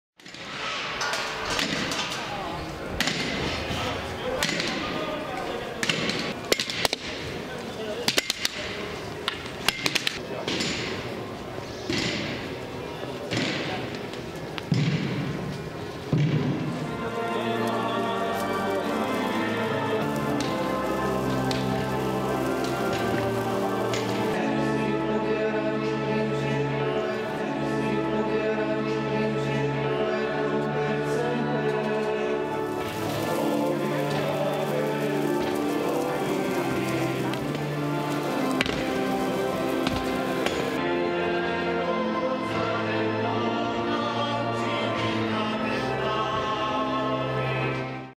Palm Sunday in Jerusalem's Holy Sepulchre
Worshippers gathered at the Church of the Holy Sepulchre in Jerusalem to mark Palm Sunday, waving palm branches during the procession next to the newly restored Tomb of Jesus.

The church, located in Jerusalem's Old City, is believed to be the burial site of Jesus Christ.